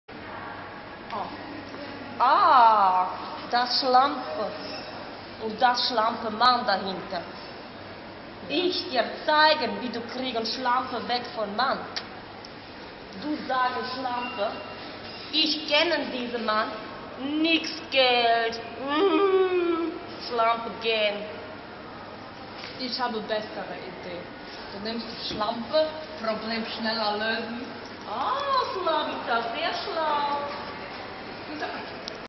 Ludmila & Slavica: Folge 2 - Schlampe weg kriegen
Ludmila (Klaudija Paunovic) und Slavica (Laila S.) haben auch dieses Mal wieder eine echte Lebensweisheit zu bieten.